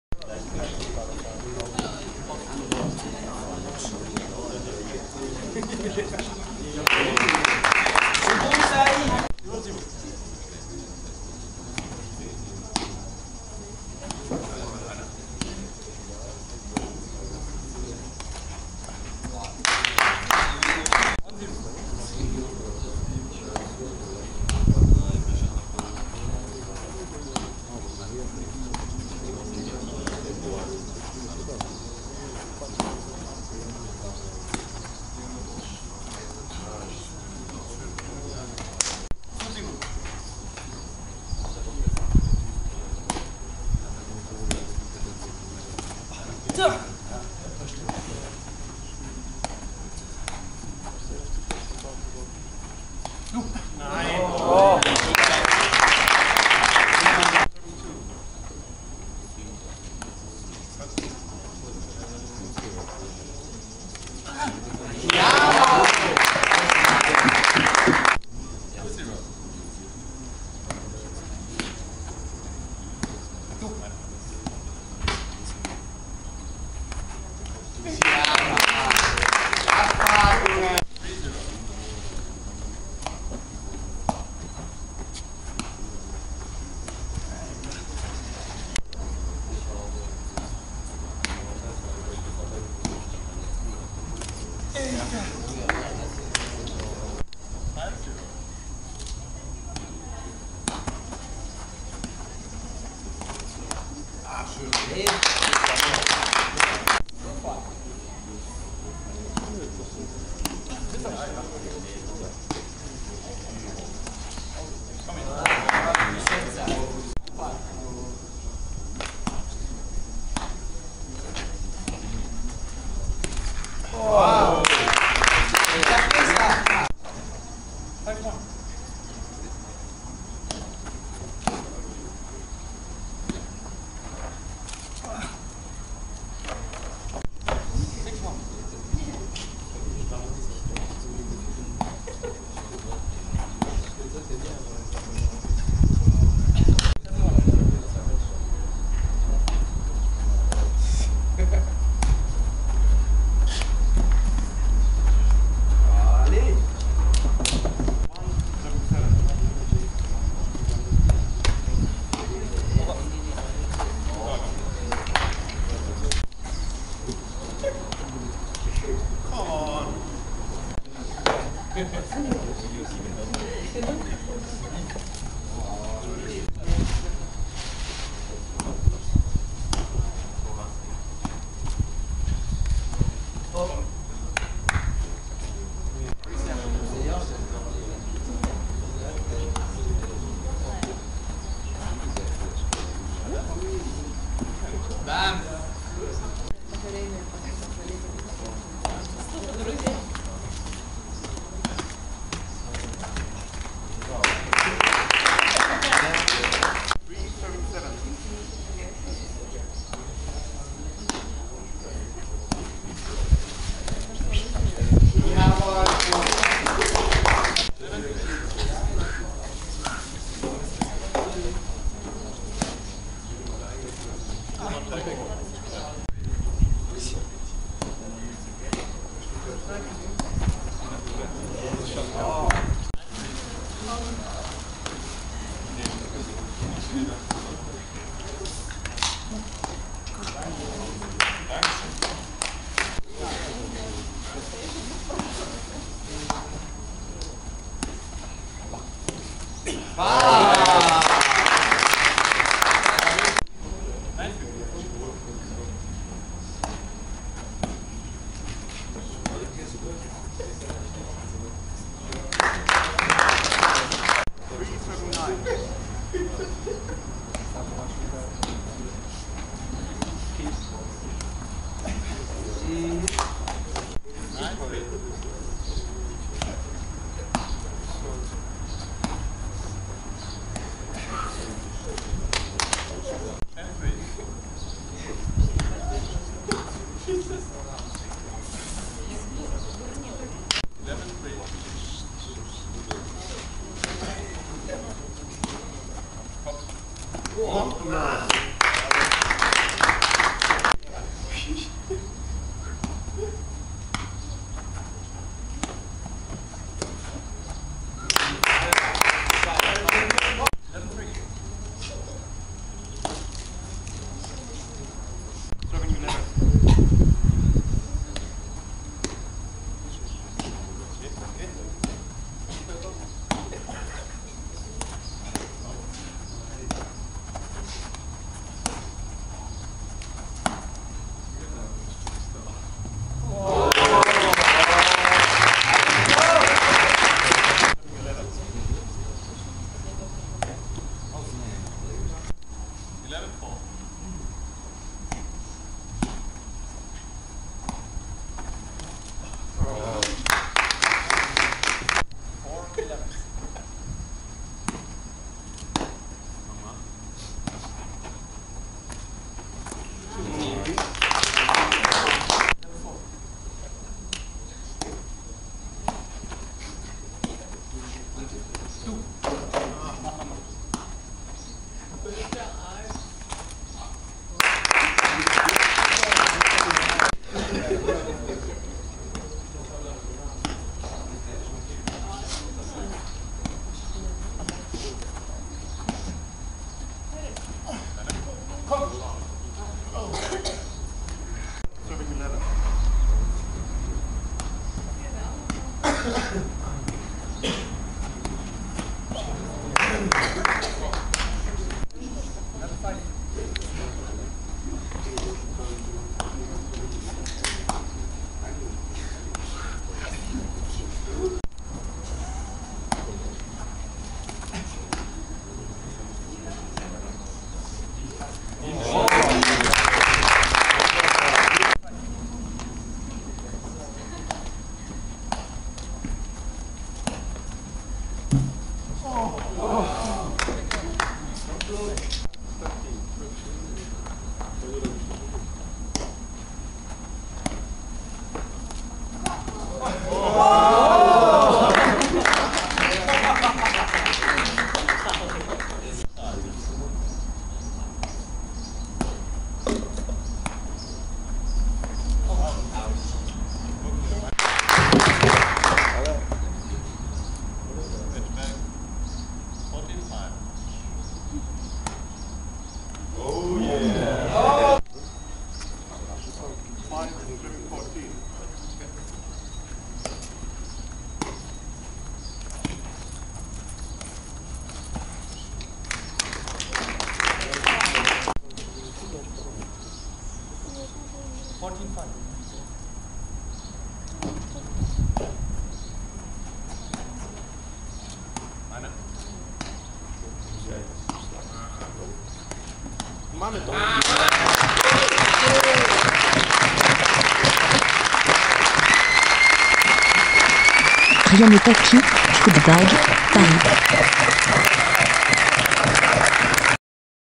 European Footbag Championships 2011 Doubles Net Final Set 2
Florian Goetze / Yves Kreil Vs Tuomas Kärki / Oskari Forsten